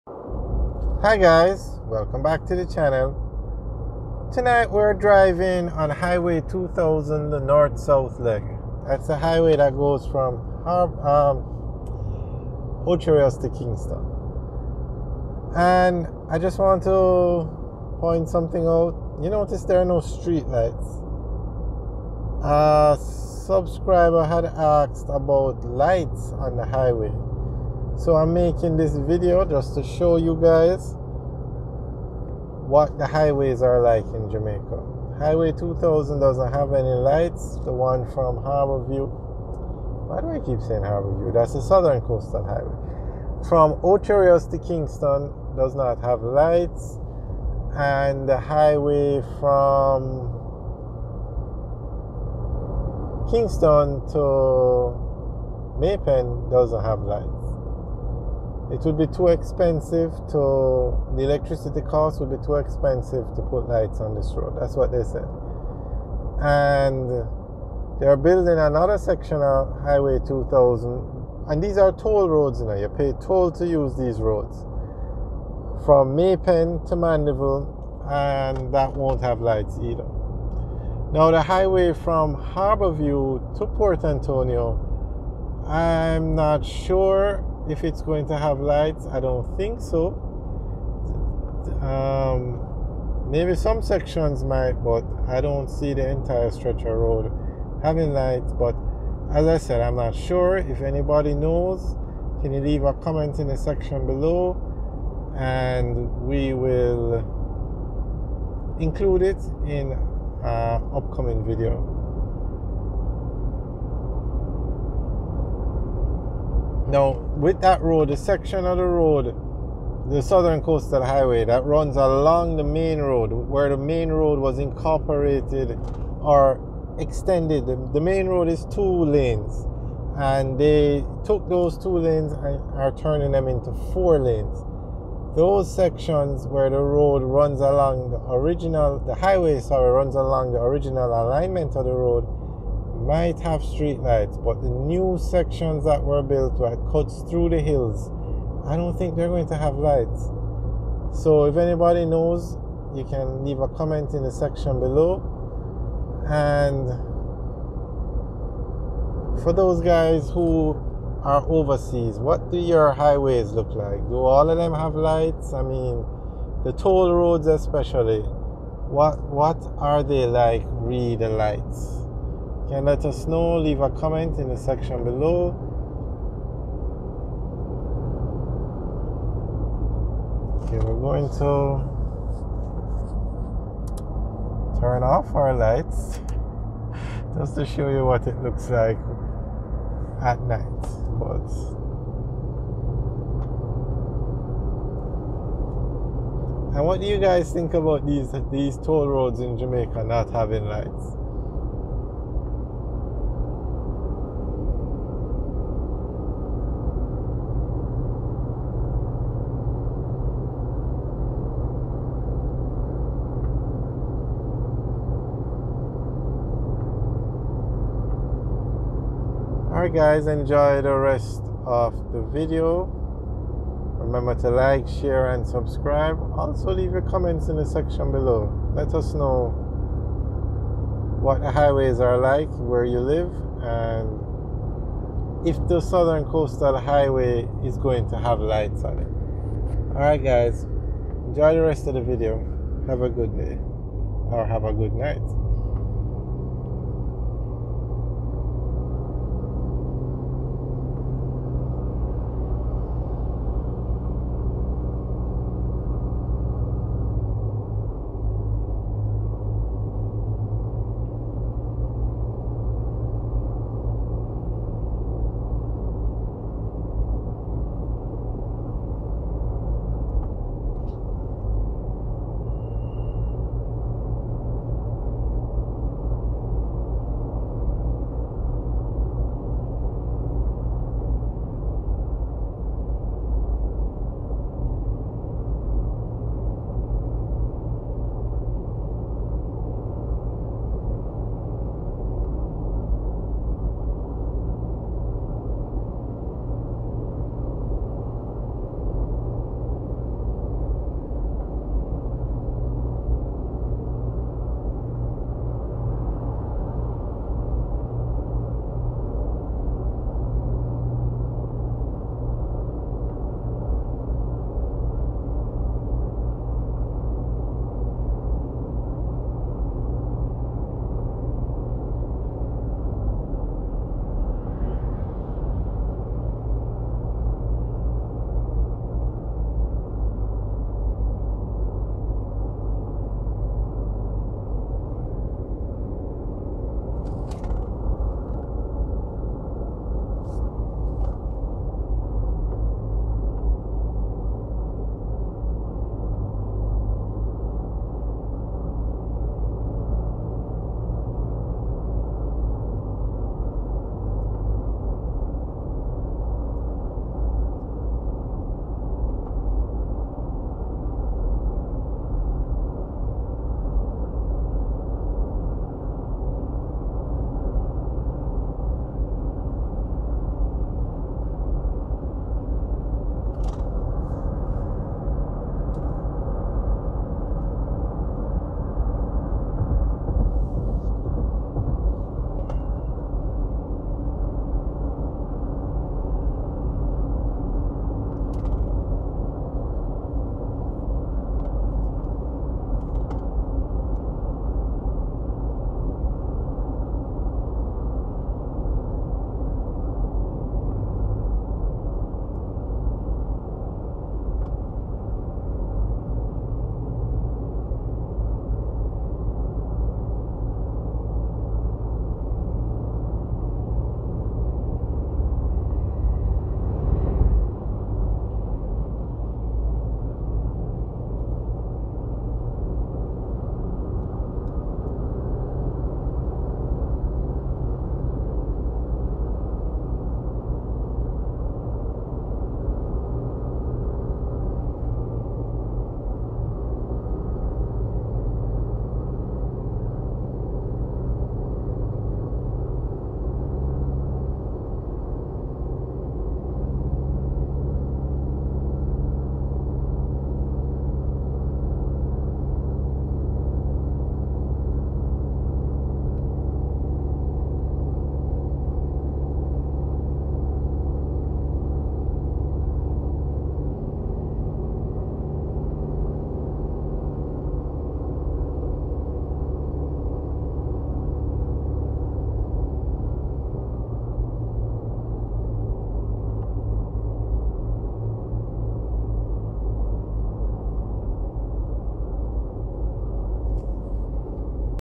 No Street Lights Jamaican Highways
A narrated driving tour No Street Lights on Jamaican Highways